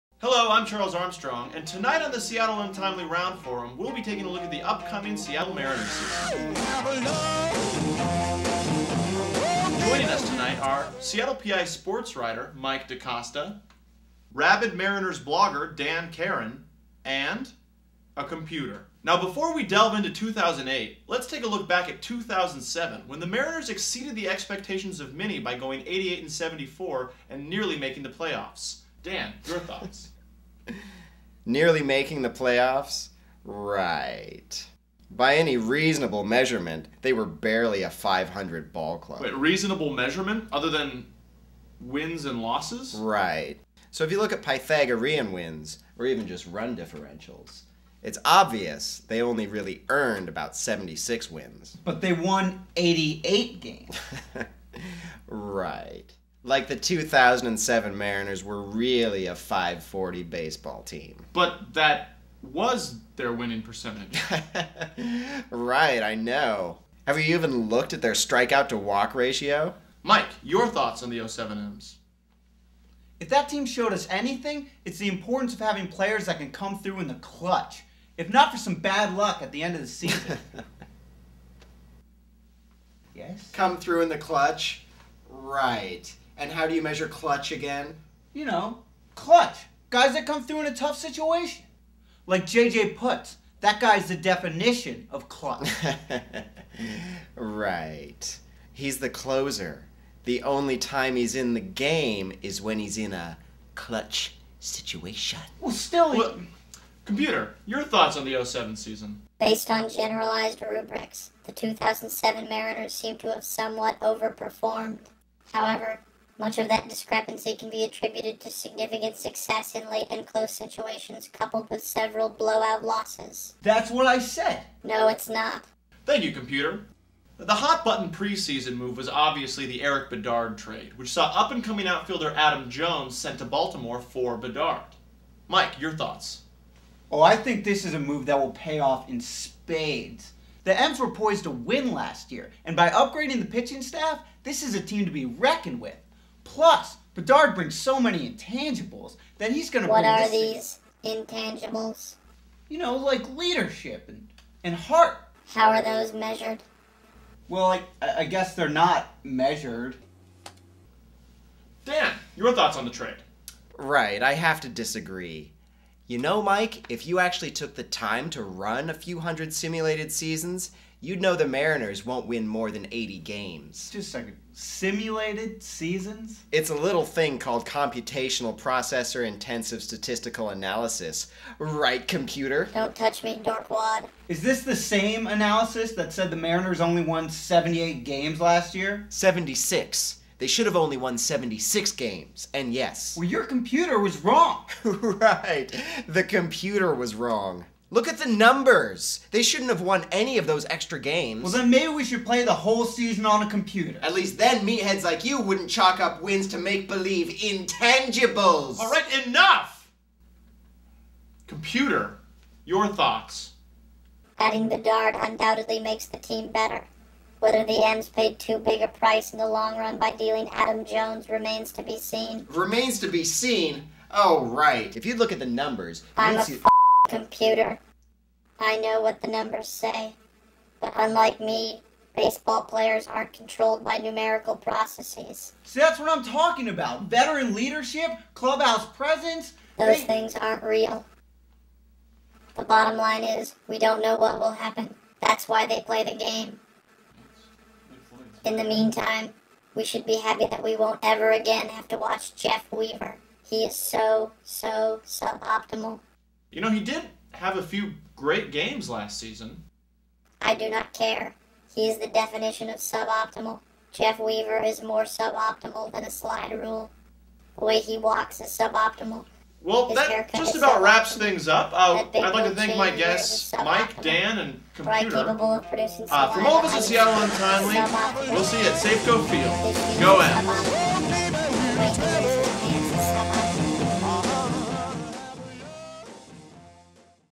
2008 Seattle Mariners Season Preview
A blogger, traditional media sportswriter, and computer give their predictions for the 2008 Seattle Mariners' season.  Special guest star Dartanion London.